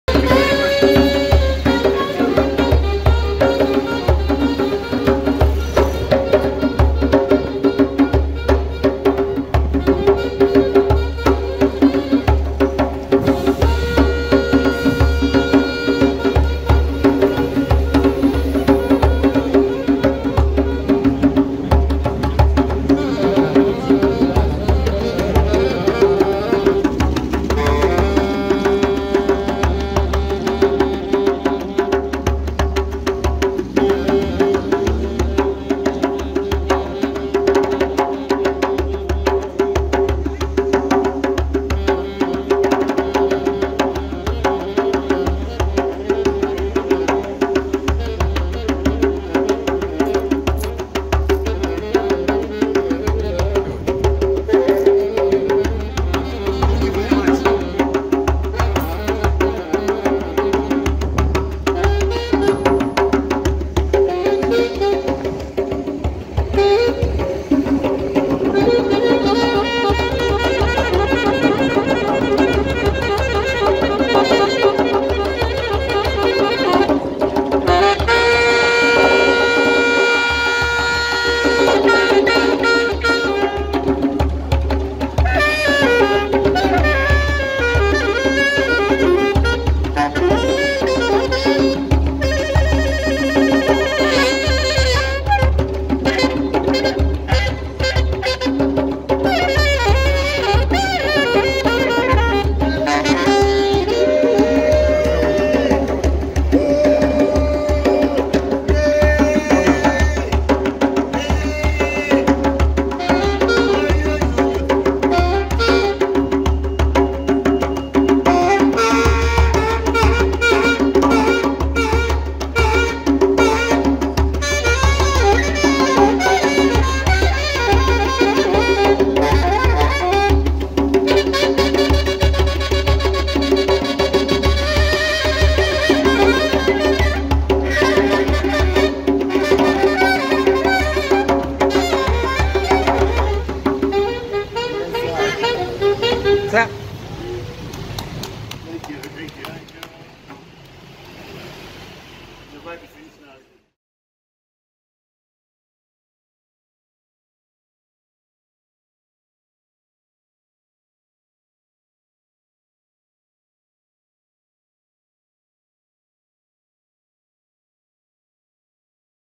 Street Entertainers | London | Shepherds Bush
Great Street performers, entertaining the public in London